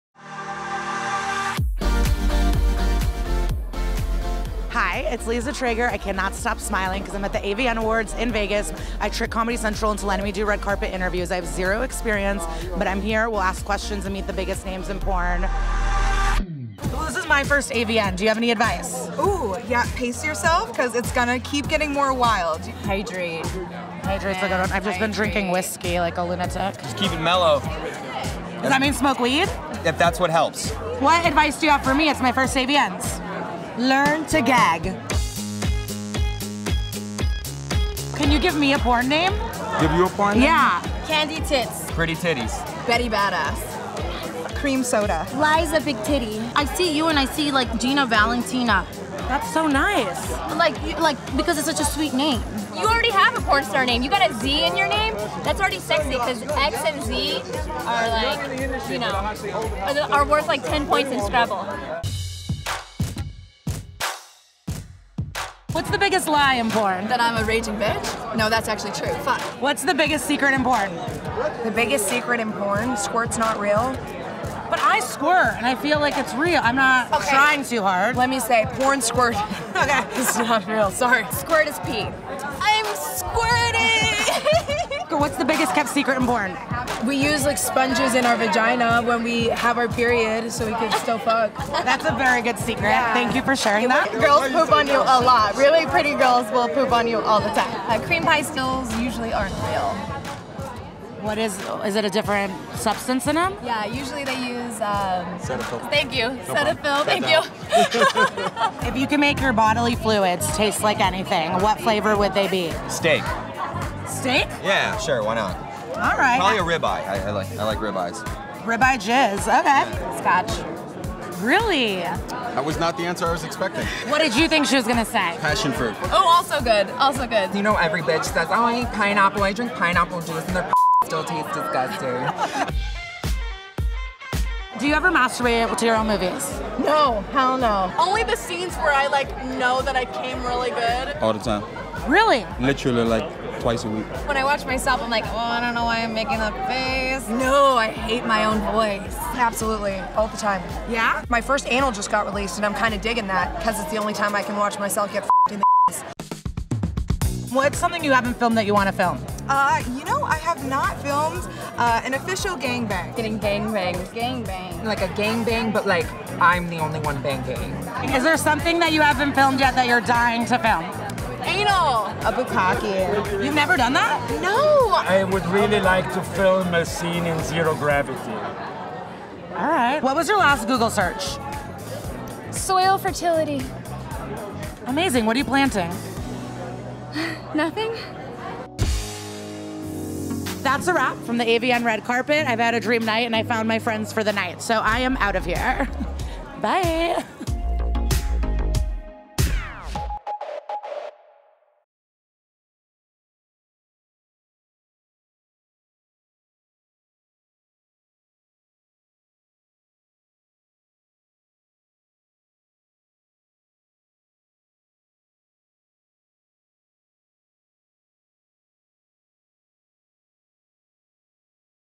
Liza Treyger Talks to Porn Stars at the AVN Awards
Liza Treyger interviews the porn stars walking the red carpet for the AVN Awards and learns some secrets of the porn industry. (Contains strong language.)